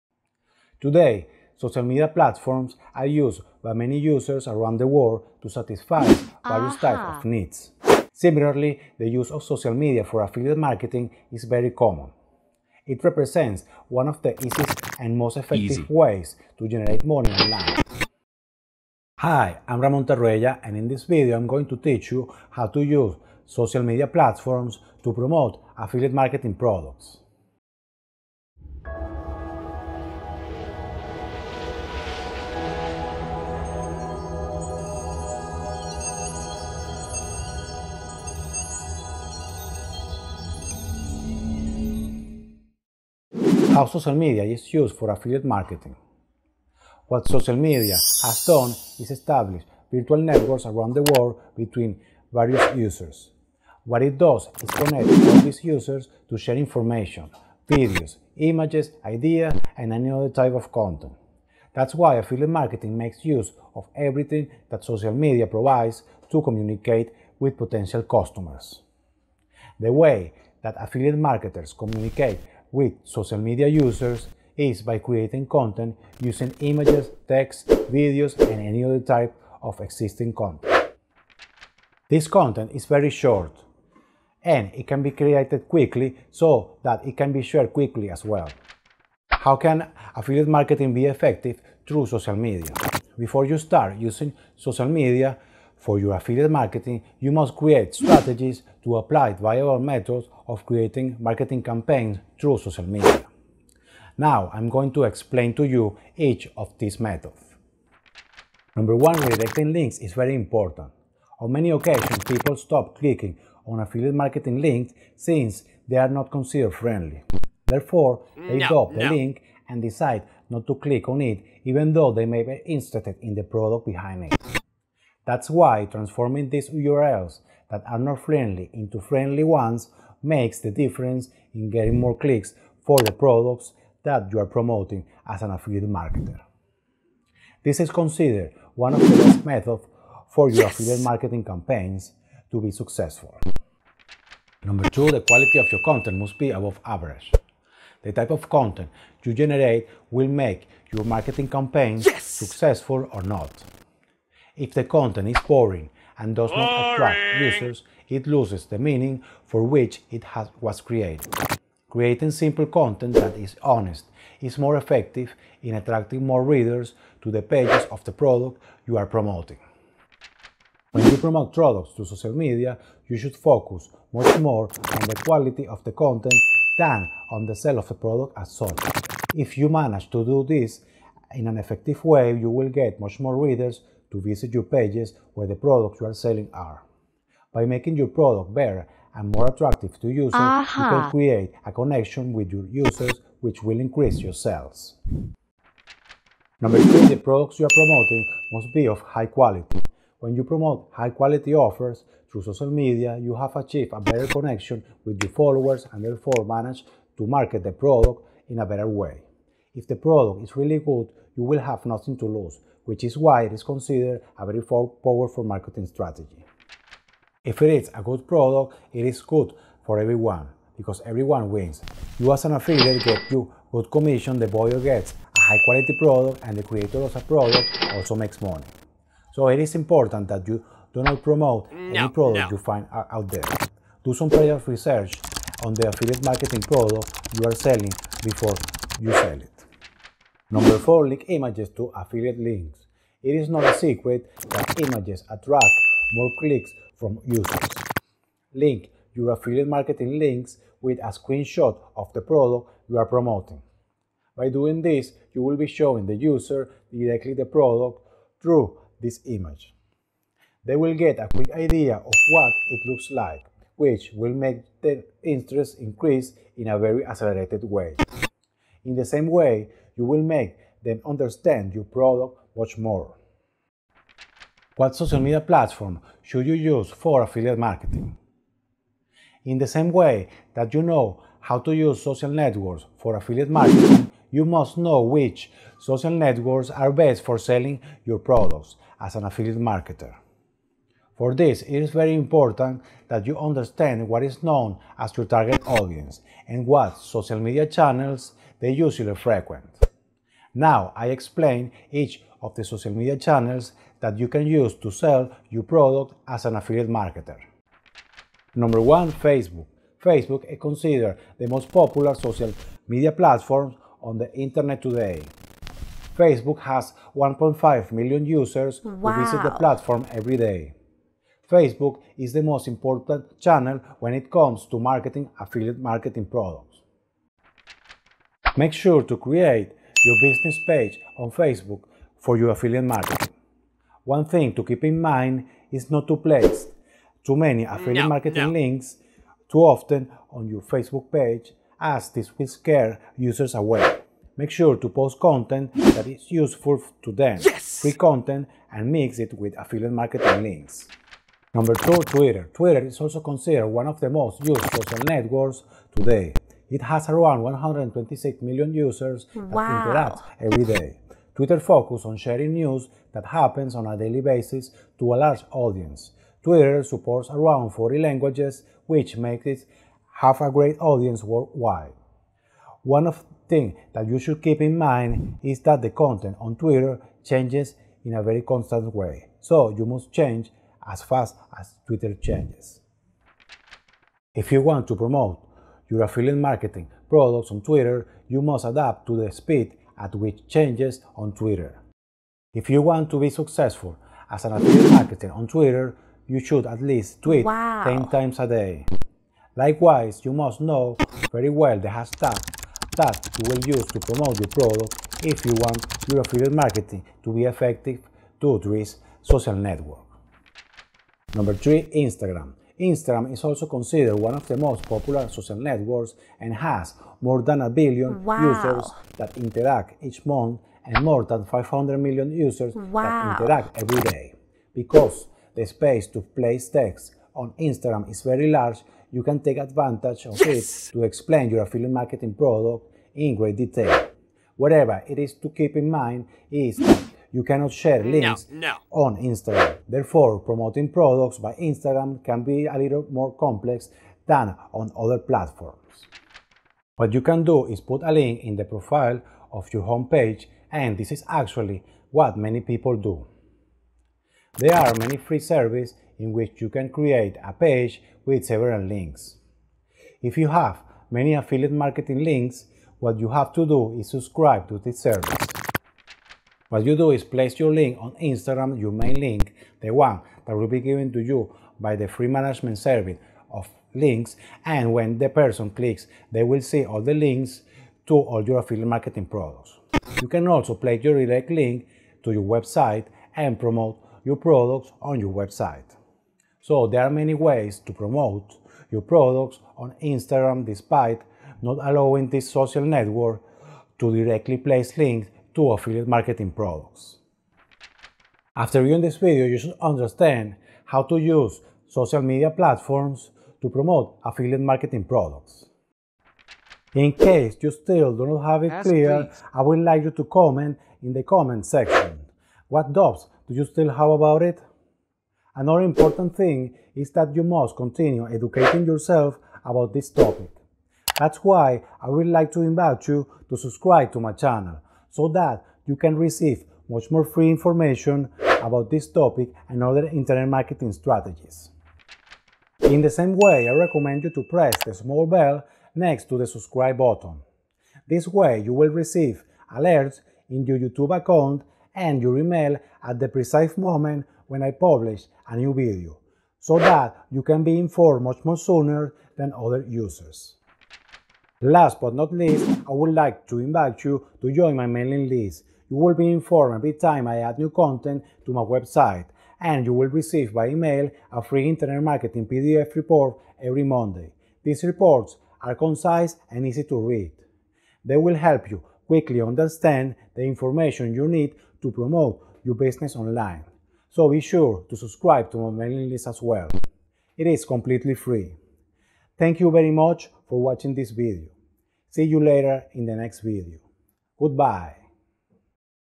How To Use Social Media Platforms To Promote Affiliate Marketing Products
Hi, in this video i'm going to teach you how to use Social Media Platforms to promote Affiliate Marketing products.

Ramon Tarruella